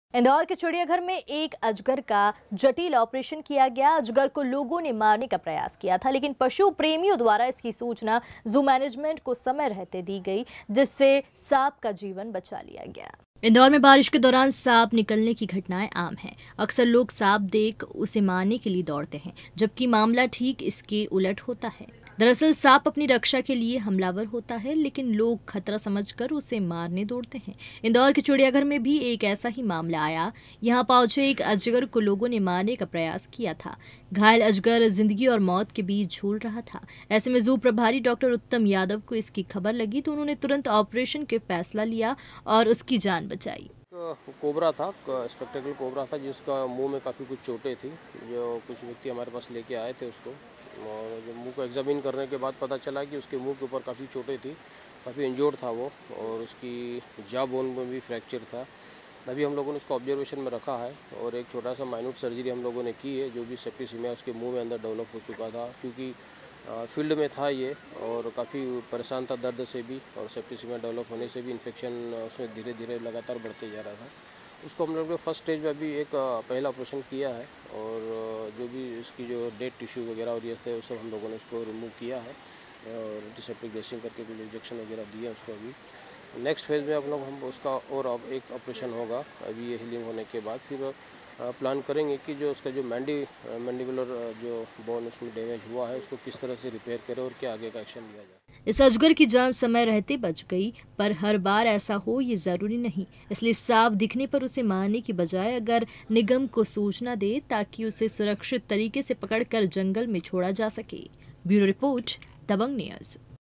INDORE घायल सांप का ऑपरेशन
इंदौर के चिड़ियाघर में एक अजगर का जटिल ऑपरेशन किया गया। अजगर को लोगों ने मारने का प्रयास किया था लेकिन पशु प्रेमियों द्वारा इसकी सूचना जू मैनेजमेंट को समय रहते दे दी गई। जिससे सांप का जीवन बचा लिया गया।
एक्टेंशन - सांप का ऑपरेशन करते हुए